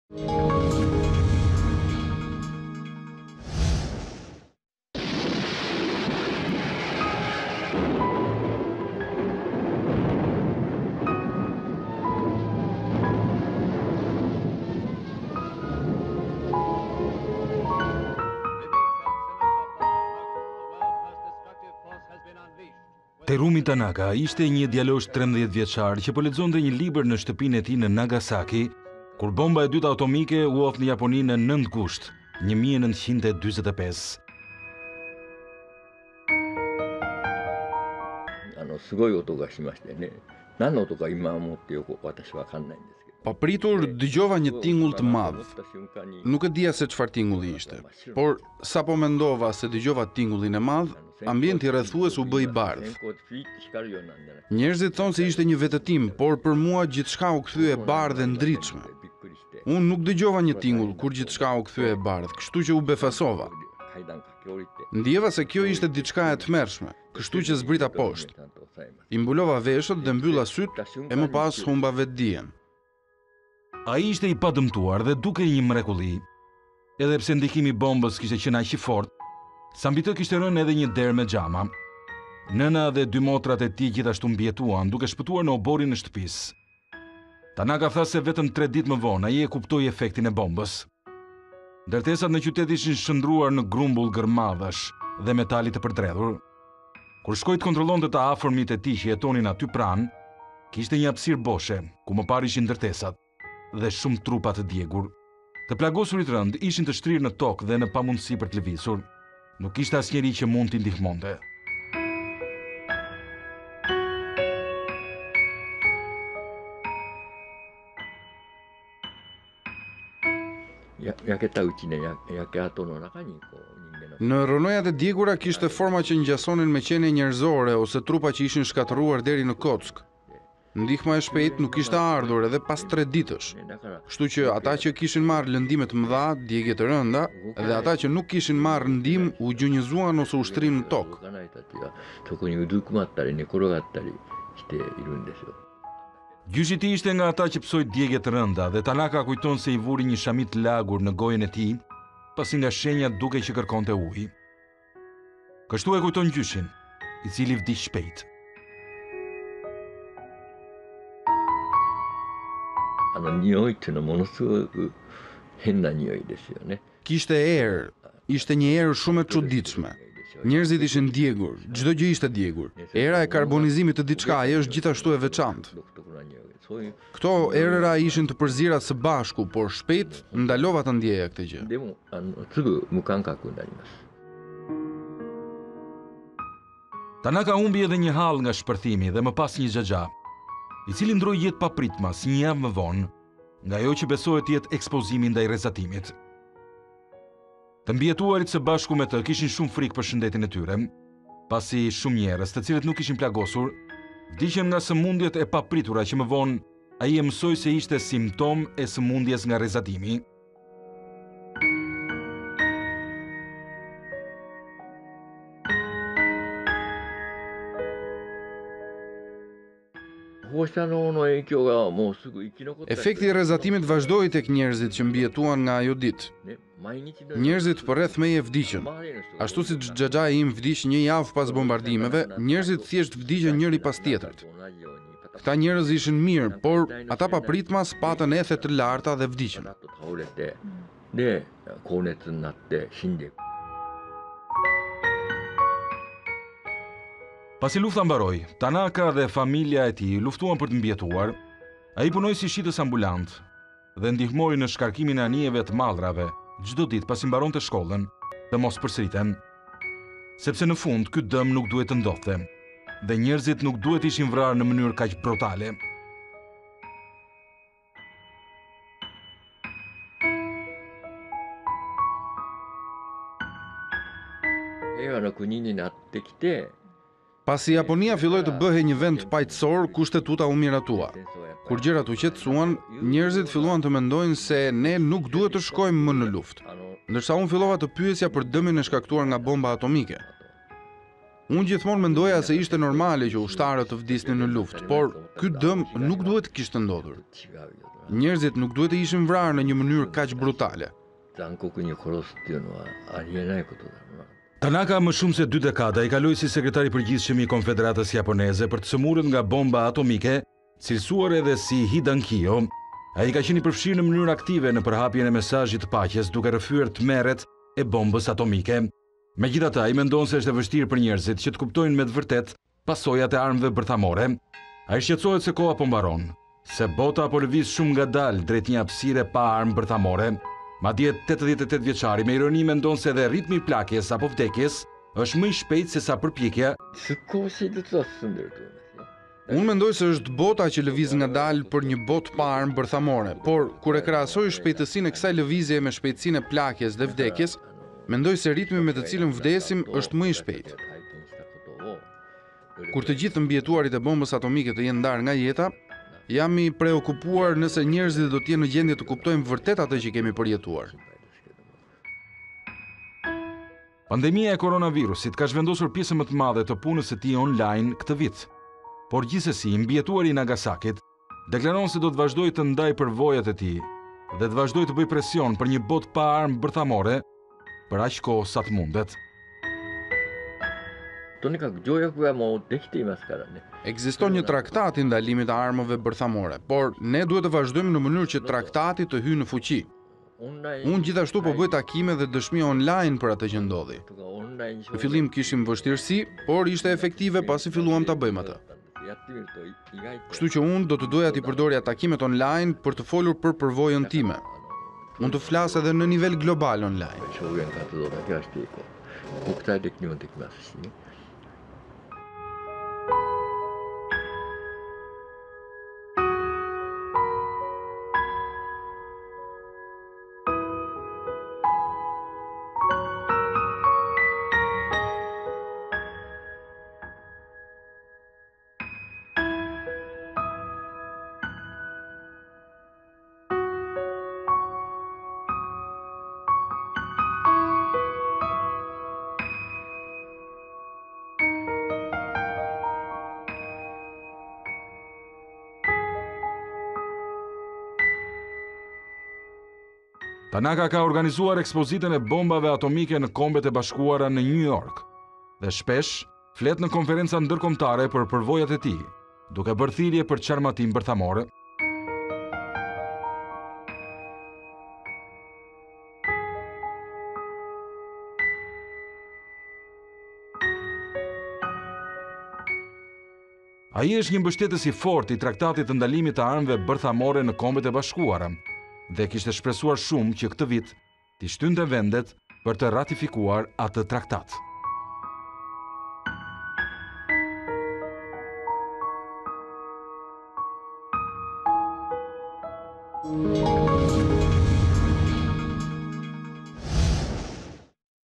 I mbijetuari i Nagasakit
Terumi Tanaka ishte një djalosh 13 vjeçar që po lexonte një libër në shtëpinë e tij në Nagasaki kur Bomba e Dytë Atomike u hodh në Japoni më 9 gusht 1945. Ai e kujton atë si një tingull me zhurmë të madhe dhe pastaj gjithçka u bë e bardhë. Ai e kujton si diçka të tmerrshme, por instikti e shtriu përtokë duke mbuluar veshët e mbyllur sytë. Edhe pse humbi vetëdijen ai shpëtoi.

“Papritur, dëgjova një tingull të madh. Nuk e dija se çfarë tingulli ishte, por sa po mendova se dëgjova tingullin e madh, ambienti rrethues u bë i bardhë. Njerëzit thonë se ishte një vetëtimë, por për mua gjithçka u kthye e bardhë e ndritshme. Unë nuk dëgjova një tingull kur gjithçka u kthye e bardhë kështu që u befasova. Ndjeva se kjo ishte diçka e tmerrshme, kështu që zbrita poshtë. I mbulova veshët dhe mbylla sytë e me pas humba vetëdijen”, rrëfen Tanaka.

Ai ishte i padëmtuar dhe dukej një mrekulli edhe pse ndikimi i bombës kishte qenë aq i fortë sa mbi të kishte rënë edhe një derë me xhama. Nëna dhe dy motrat e tij gjithashtu mbijetuan duke shpëtuar në oborrin e shtëpisë. Tanaka tha se vetëm tre ditë më vonë ai kuptoi efektin e bombës.

Ndërtesat në qytet ishin shndërruar në grumbull gërmadhash dhe metali të përdredhur. Kur shkoi të kontrollonte të afërmit e tij që jetonin aty pranë, kishte një hapësirë boshe ku më parë ishin ndërtesat dhe shumë trupa te djegur. Të plagosurit rëndë ishin të shtrire në tokë dhe në pamundësi për të lëvizur. Nuk kishte asnjeri që mund t’i ndihmonte.